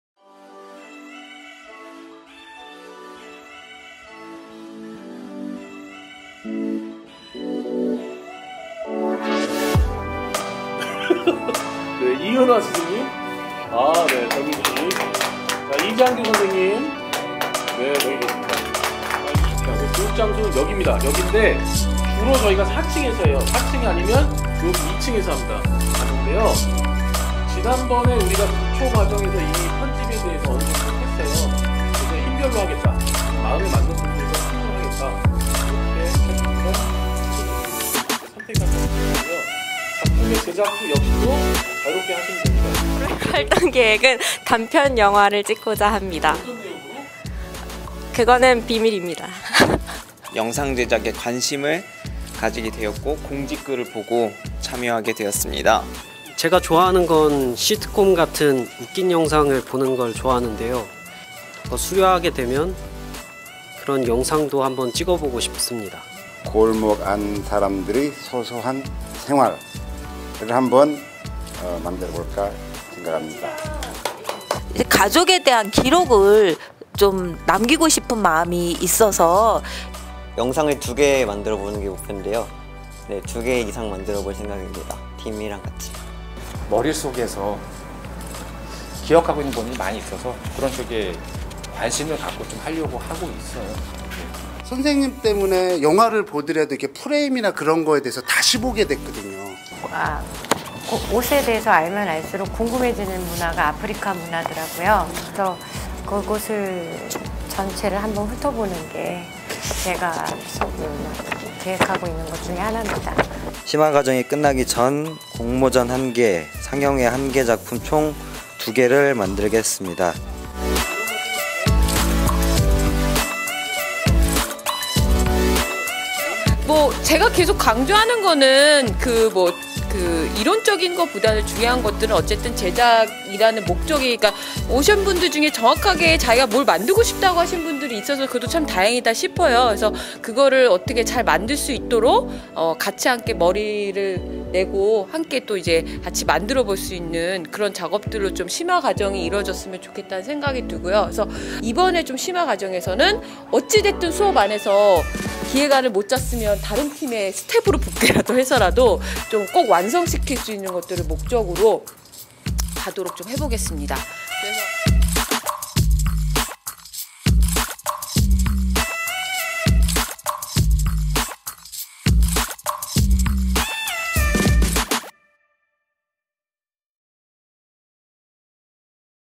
[와보숑이 만난 풍경41] 꿈꾸는 마을미디어로의 여행 {심화과정 오리엔테이션}
8월16일에 진행된
와보숑 영상편집 심화과정 신청자들과 함께한 오리엔테이션.

올해는 또 어떤 영상이 나올 지, 기대해 봅니다.

# 가을에 하게 될 영상상영회도 많이 기대해 주세요.

기획,제작: 미디어협동조합 와보숑